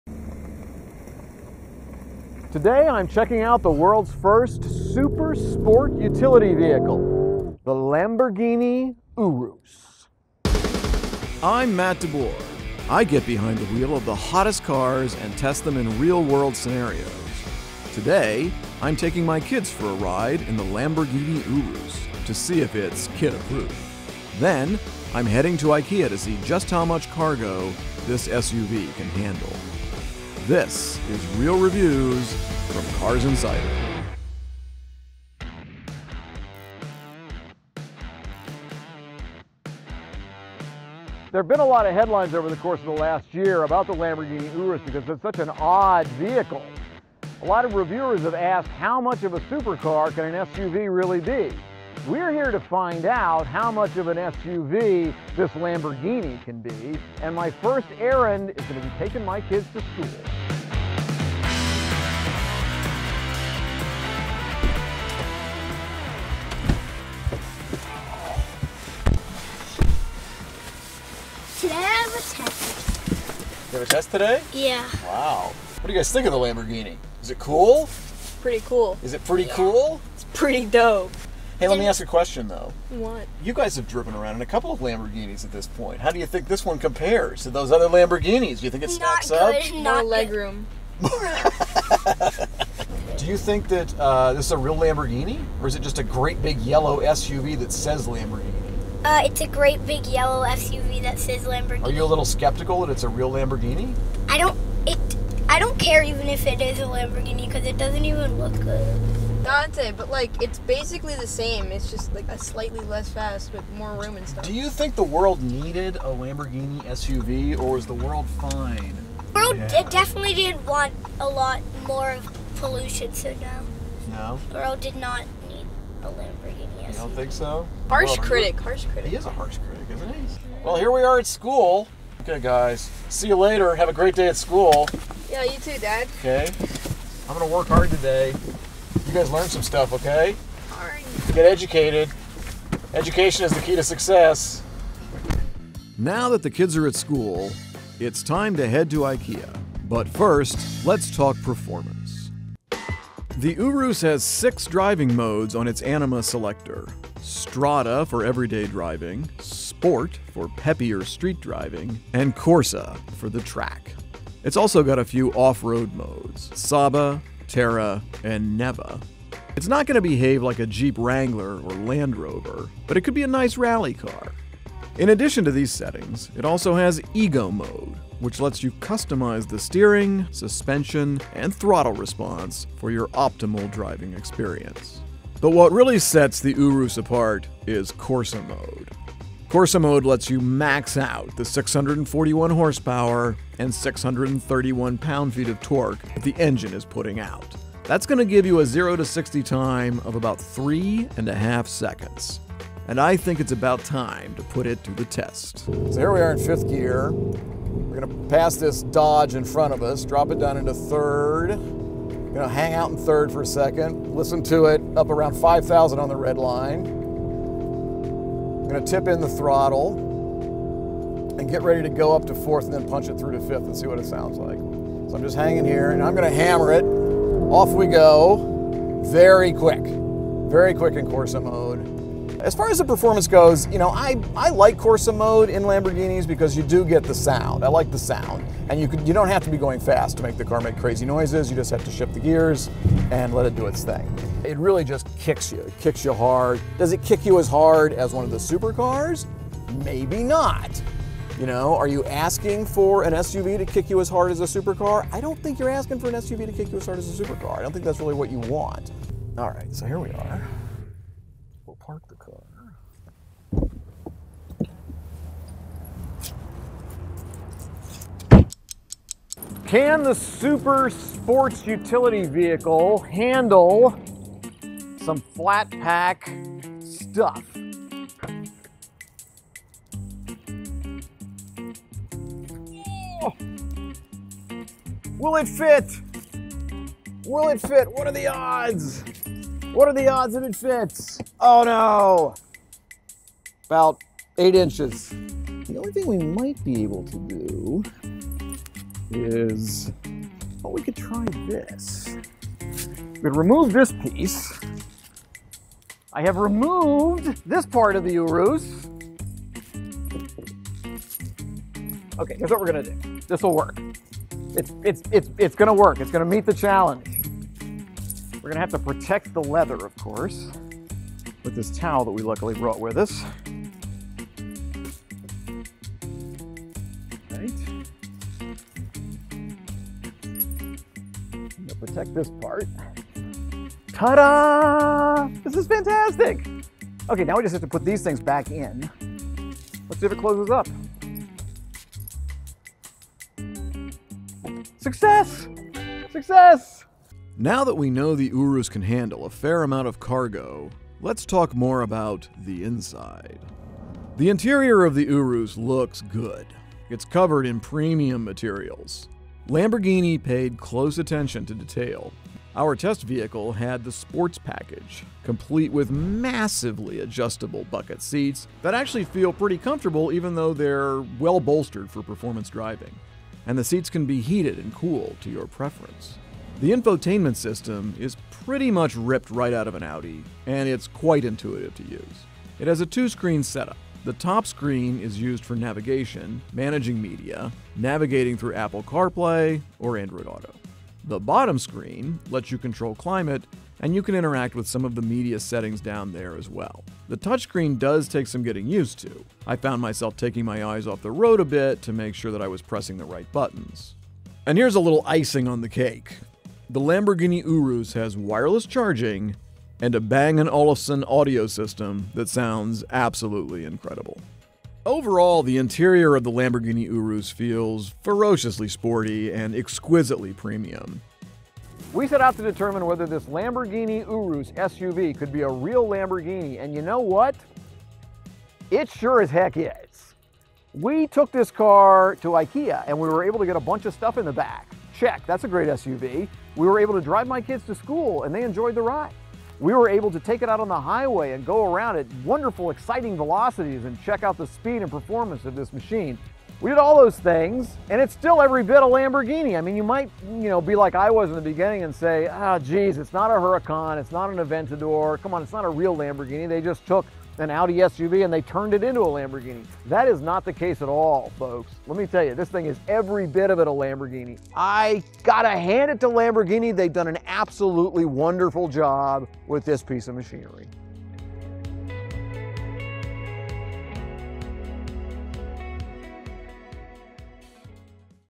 Can The Lamborghini Urus Act As A Normal Family SUV? | Real Reviews
Lamborghini made its first SUV since 1986 — the Urus. It's labeled as a "super" SUV. With supercar-like performance, we reviewed the Urus to see if it still holds up as a practical SUV.